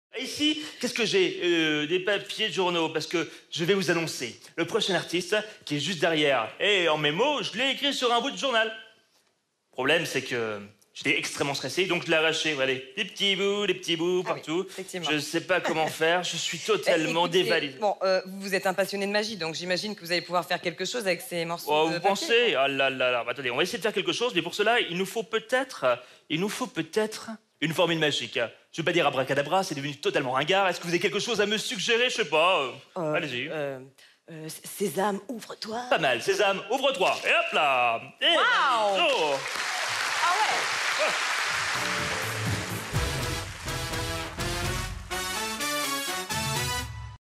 Mandrakes d'Or 2018 : Maxime Guény réalise un tour de magie du journal déchiré puis reconstitué
Dans le cadre des Mandrakes d'Or, Maxime Guény réalise le tour du journal déchiré, puis reconstitué.